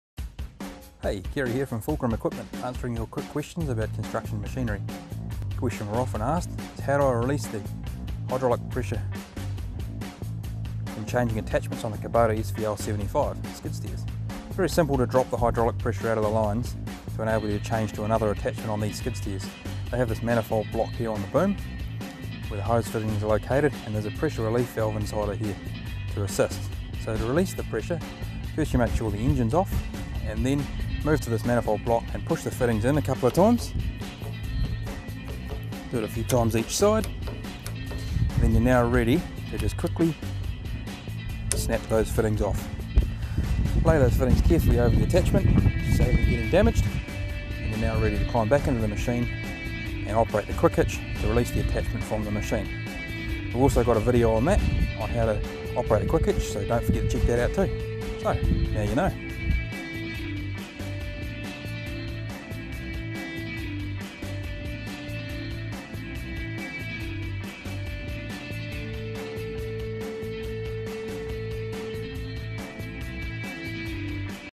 How do I release the quick coupler fittings on my Kubota Skid steer attachment?
Hey, Kerry here from Fulcrum Equipment, answering your Quick Questions about construction machinery.
A question we’re often asked – How do I release hydraulic pressure when changing attachments on a Kubota SVL skid steer?
It’s very simple to drop the hydraulic pressure out of an attachment to enable you to change to another attachment on these skid steers.  They have a manifold block on the boom where the hose fittings are located and there is a pressure relief inside this manifold.
So to release the pressure you first make sure the engine is turned off, then push the fittings back into the manifold, several times each side.  What this does is open up a small hole which allows any built up pressure to go back to the hydraulic tank.
Now you can slide the outer ring on the female quick coupler back and the fitting will release, repeat with the other fitting and don’t forget to unplug any electrical connectors as well.  Lay the hoses and any cables carefully on the attachment where they will not get damaged, and you are ready to get back into the machine and operate the quick hitch to release the attachment itself – and by the way we’ve got a video on that too so don’t forget to check that out!
So... now you know!
Please Note: The information given in this video is general in nature. For detailed safety information please refer to the relevant manufacturer’s handbook.